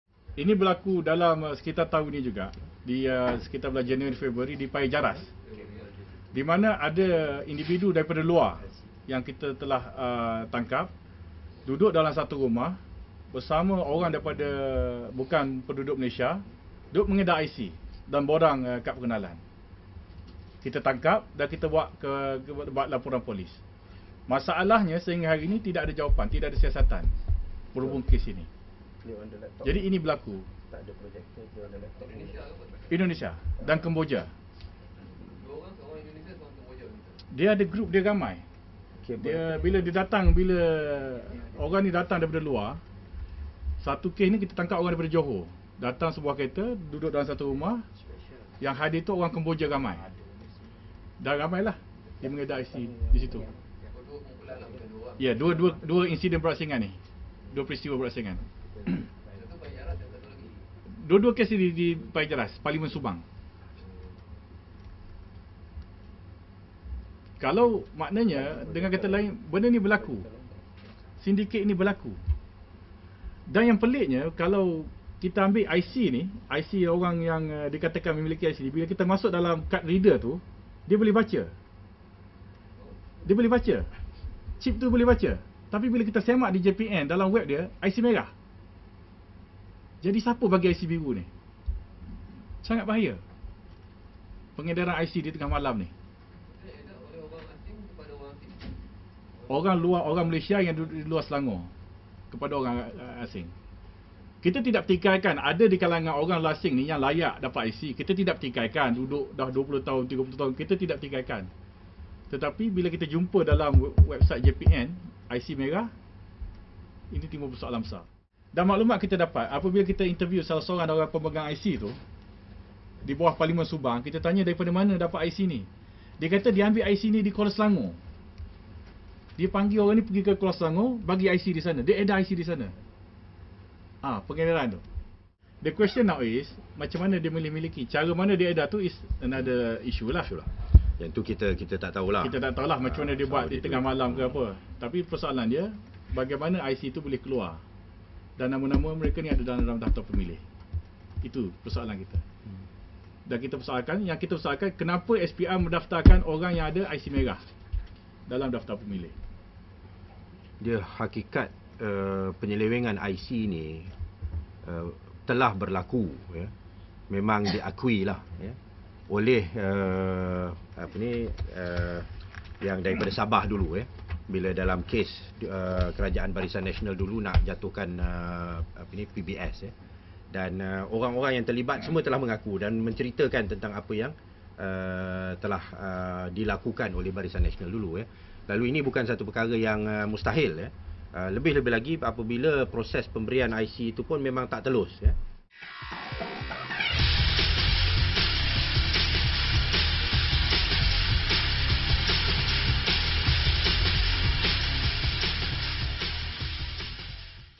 PAS S'gor dakwa tangkap sindiket edar IC
PAS Selangor mendakwa pernah tersua dengan satu kumpulan sindiket yang didakwa mengedar kad pengenalan kepada warga asing dalam satu kejadian beberapa bulan lalu.

Setiausahanya Khairuddin Othman berkata, pihaknya menyerbu dua rumah berasingan di Paya Jaras, Subang dalam satu operasi yang dijalankan bagi menangkap sindiket berkenaan.

Khairuddin mendakwa, "tuan rumah" berkenaan sedang mengedarkan kad pengenalan kepada sekumpulan yang dipercayai warga Indonesia dan Kemboja ketika serbuan dijalankan sekumpulan ahli PAS Selangor.

Camera/Editing: Tan Jiun Wuu